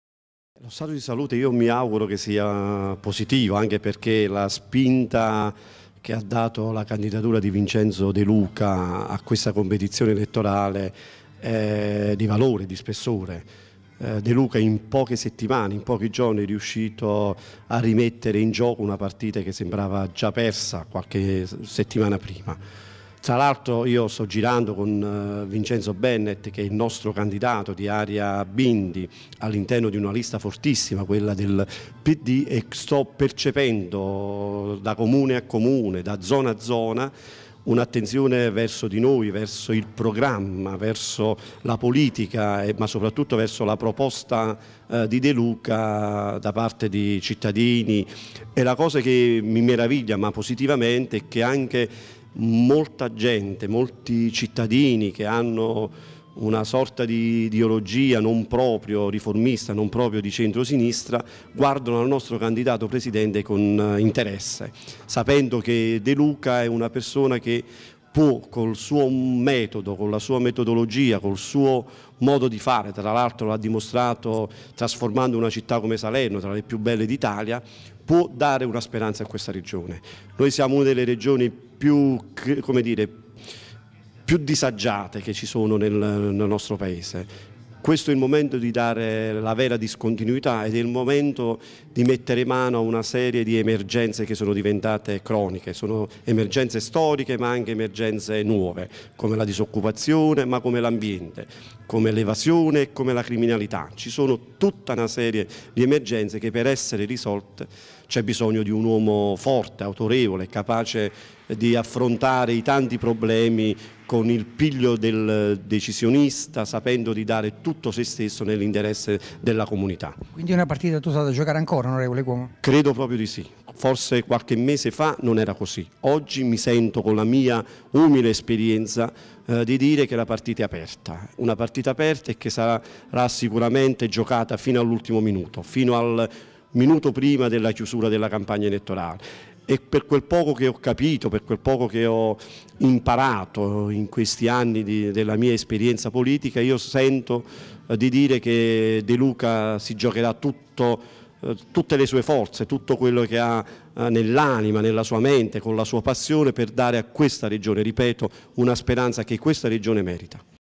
Italia 2 Tv 24/3/2010 Intervista all'on. Tonino CUOMO.mp4
Quali sono le condizioni di salute del Pd in provincia di Salerno?